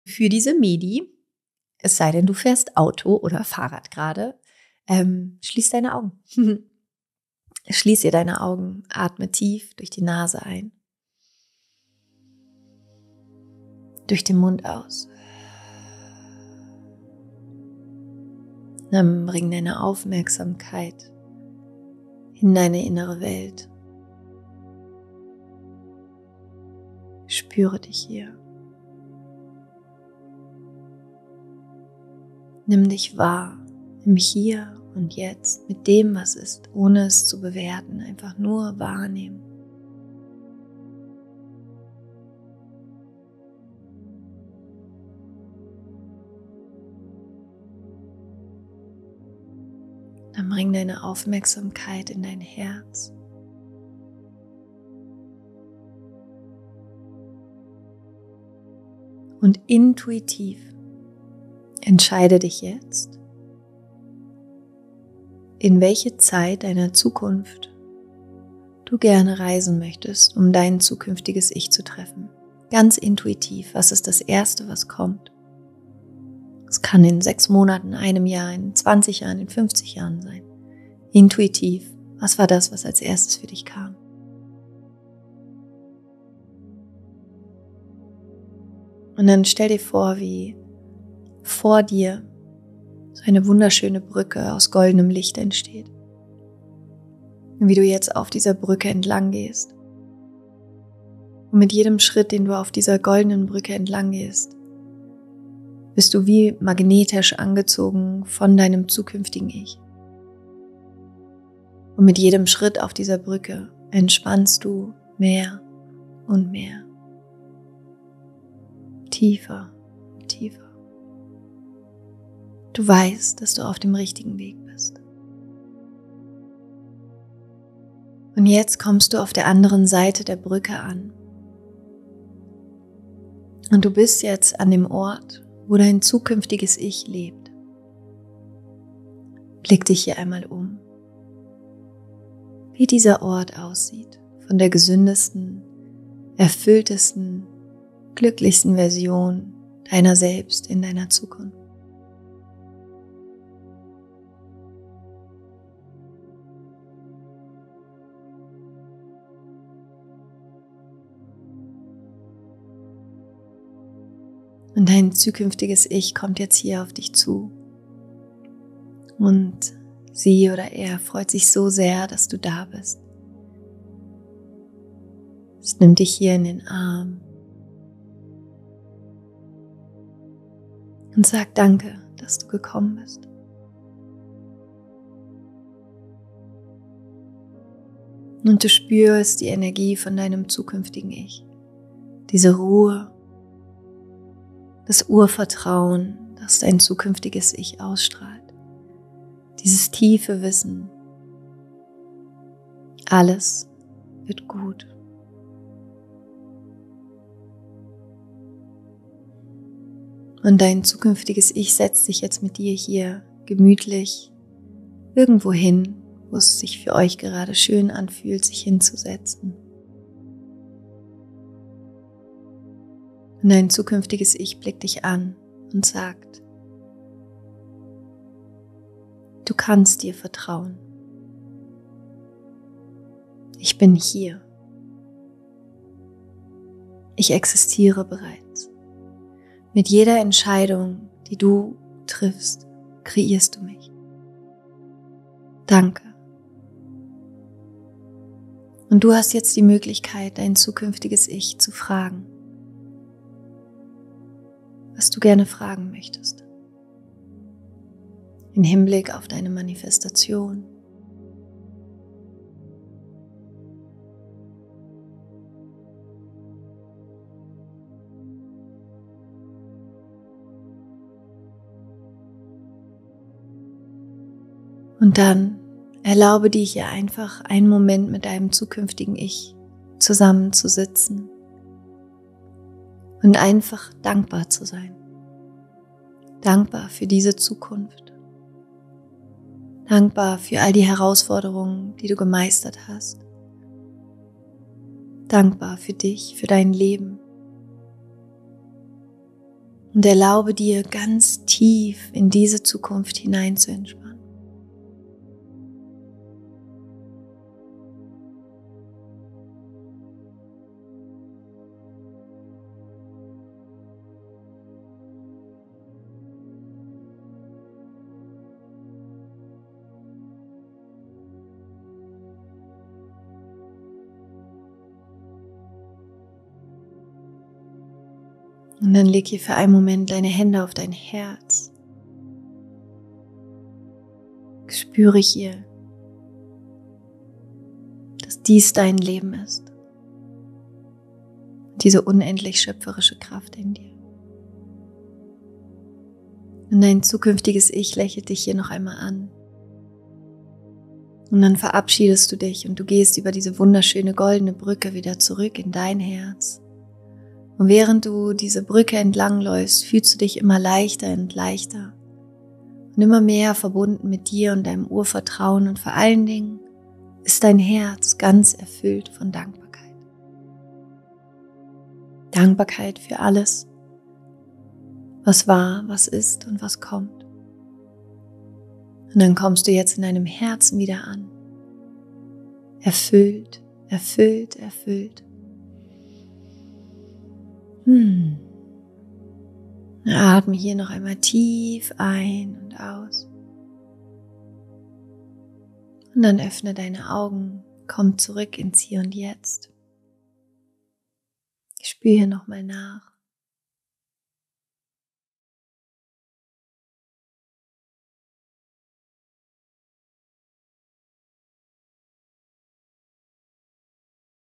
Meditation zur Verbindung mit deinem zukünftigen Ich
In dieser 5-minütigen Meditation nutzt du einen meiner powervollsten Manifestationshacks!

Die Meditation ist Teil meiner Podcastfolge: Manifestation Special #4: Lerne meinen powervollsten Manifestastionshack! Höre sie dir unbedingt an, wenn du erfahren möchtest, wie ich mit meinem Future Self manifestiere.

In dieser Meditation erwartet dich…
✨ ein Zeitsprung in deine Zukunft
✨ ein Rat von deinem zukünftigen Ich
✨ ein tiefes Vertrauen in dich und deine Ziele
✨ eine Unterstützung für schwierige Entscheidungen
✨ Klarheit, Vertrauen, Liebe und das Wissen, dass alles gut sein wird.

Ich wünsche dir eine wunderschöne Begegnung mit deinem zukünftigen Ich!

Schreib mir gerne in die Kommentare, welchen Ratschlag dein zukünftiges Ich für dich hatte.

Rock on & Namasté
Deine Laura

Hinweis: Die bereitgestellten Inhalte dienen ausschließlich Informations- und Bildungszwecken und stellen keinen Ersatz für therapeutische und medizinische Beratung dar.